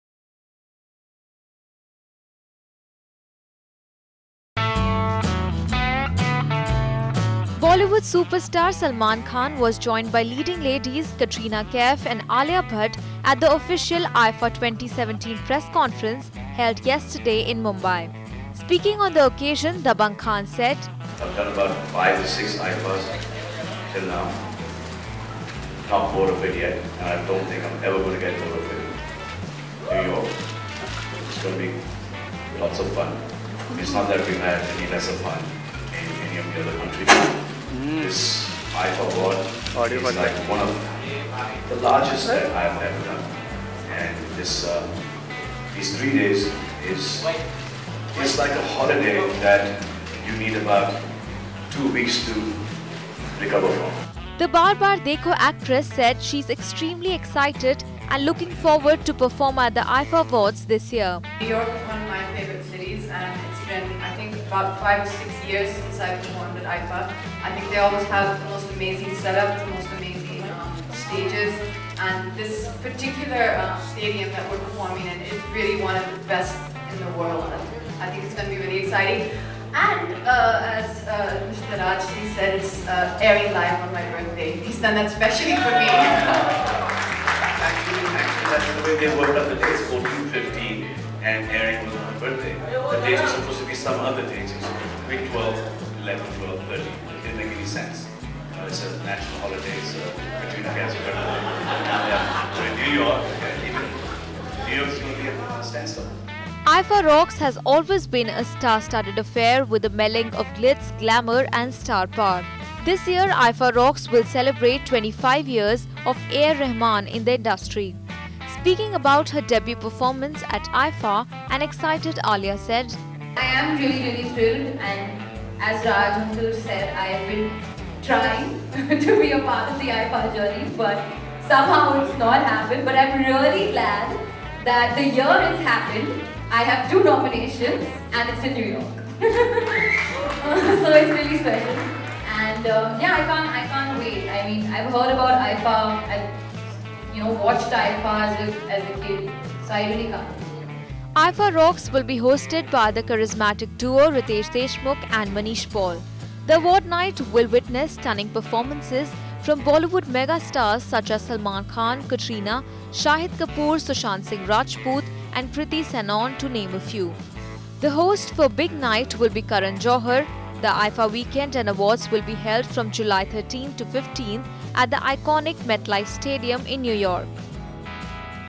Watch: Salman, Katrina, Alia dazzle at IIFA 2017 press conference - Bollywood News
Mumbai (Maharashtra) June 02 (ANI): Bollywood superstar Salman Khan was joined by leading ladies Katrina Kaif and Alia Bhatt at the official IIFA 2017 press conference held yesterday in Mumbai. The trio shared their excited and said and they are looking forward to perform at the IIFA Awards this year. IIFA Rocks has always been a star-studded affair with a melange of glitz, glamour and star power. Hosted by Karan Johar, the award night will witness stunning performances from Bollywood megastars such as Salman, Alia, Katrina, Shahid Kapoor, Sushant Singh Rajput and Kriti Sanon, to name a few. The star-studded award night will be held from July 13-15 at the iconic MetLife Stadium in New York.